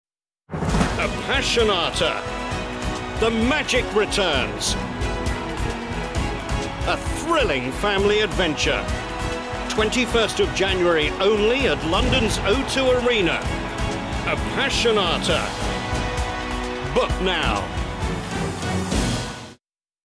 Apassionata at the O2 Arena Trailer
This magical horse show returns to the O2 Arena for two performances only in January 2012.